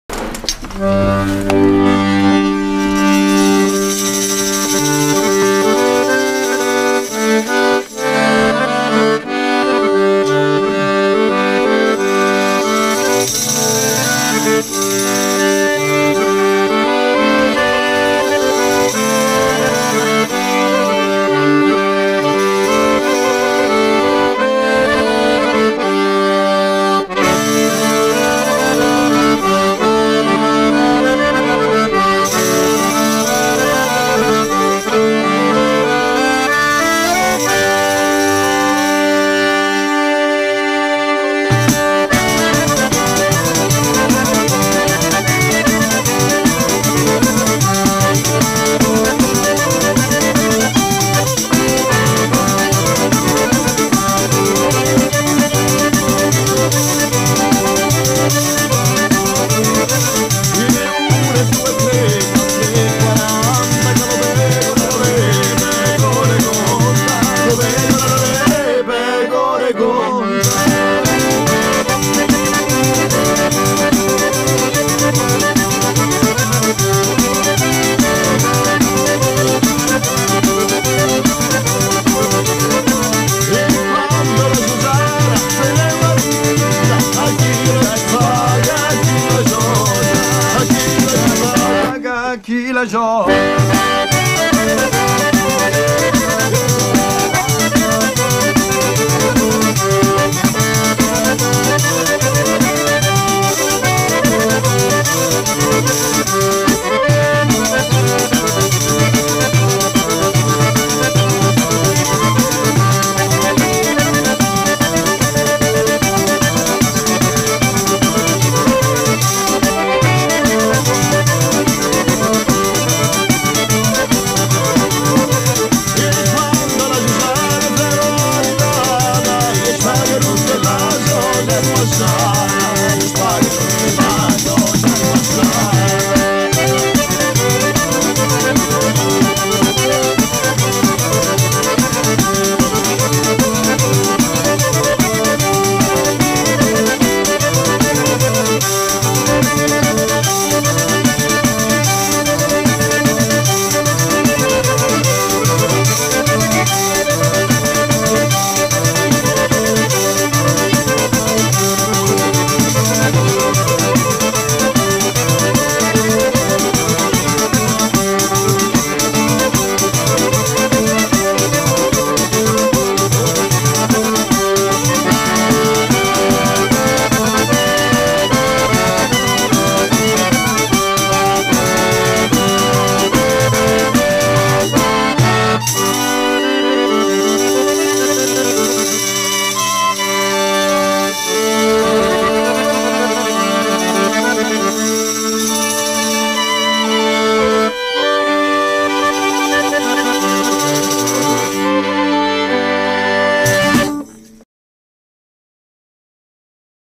Radici Popolari
Italian Folk Band Radici Popolari have been wowing the crowds at the Errigal Arts festival - he they perform live on the Weekend edition!